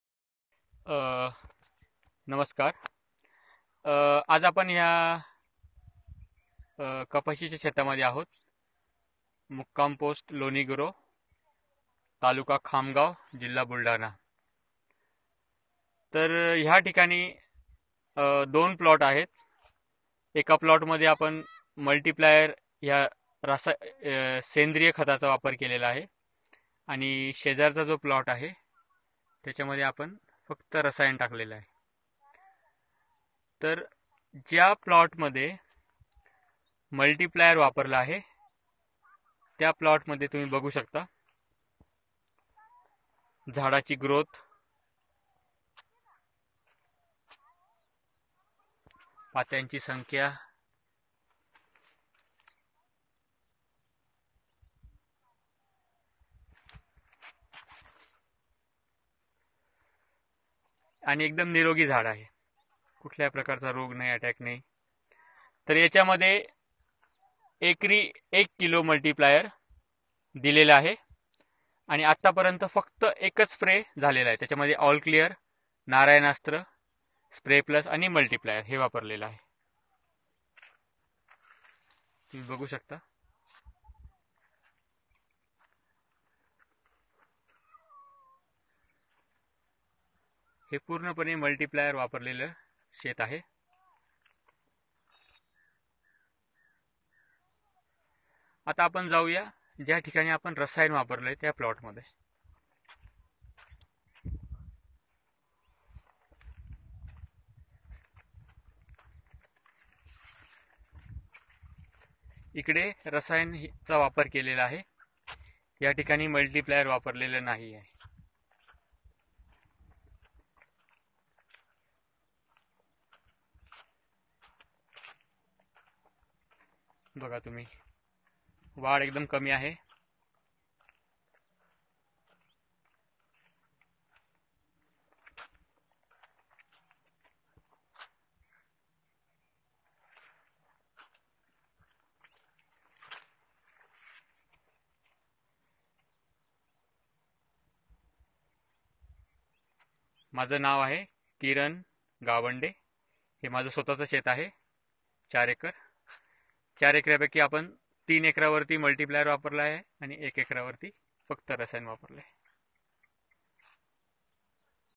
मल्टीप्लायर तकनीक के साथ कपास की खेती
कपास की खेती.
मल्टीप्लायर तकनीक के साथ.

१) किसान भाई का नाम श्री किरण नामदेव गावंडे ग्राम लोनी गुरव तहसील खामगांव जिला बुलढाणा.
२) मार्गदर्शक तथा मल्टीप्लायर विक्रेता किरण सर स्वयं मल्टीप्लायर विक्रेता हैं.
३) कपास की खेती में मल्टीप्लायर तकनीक का इस्तेमाल किया.
४) किरण सर ने एक प्लाट में मल्टीप्लायर तकनीक का इस्तेमाल किया है, दूसरे प्लाट में सिर्फ रासायनिक खाद डाला है.
५) मल्टीप्लायर तकनीक इस्तेमाल किये खेत में रासायनिक खाद 30 प्रतिसत कम डाला है, उसके बावजूद पौधों की ग्रोथ और शाखाओं की संख्या में मल्टीप्लायर वाली फसल दुगनी से ज्यादा आगे है.
६) जिस फसल में मल्टीप्लायर का इस्तेमाल नहीं हुआ है, वह फसल बहोत छोटी है.